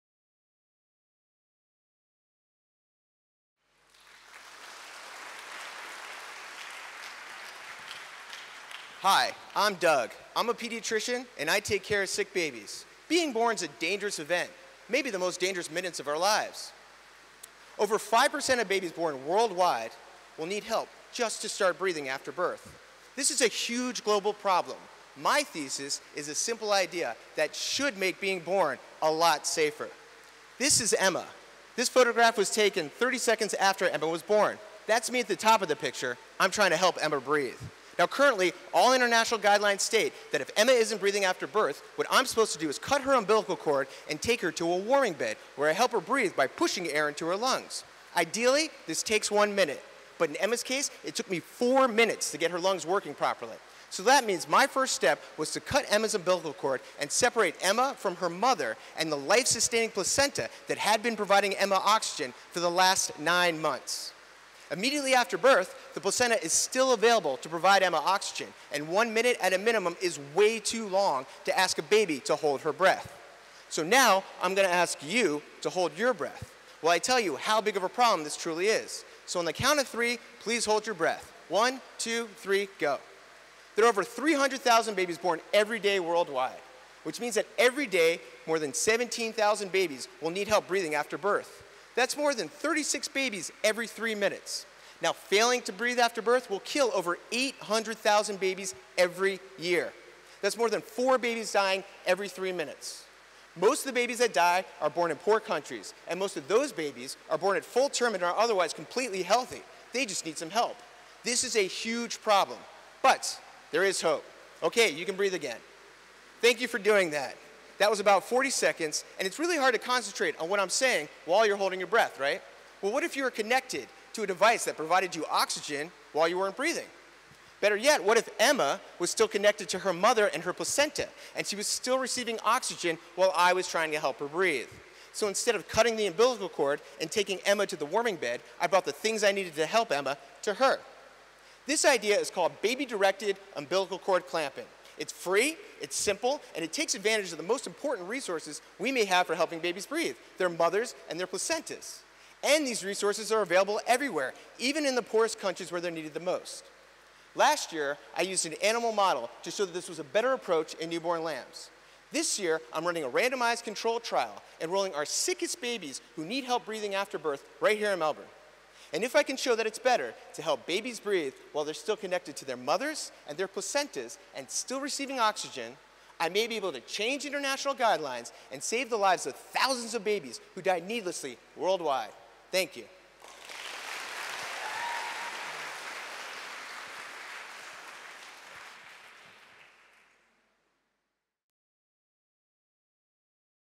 Dr Douglas Blank - Monash 3MT People's Choice Award Winner 2016
Dr Douglas Blank
Faculty of Medicine, Nursing and Health Sciences
3MT Title: Baby-directed umbilical cord clamping: Improving the way we help babies breathe after birth.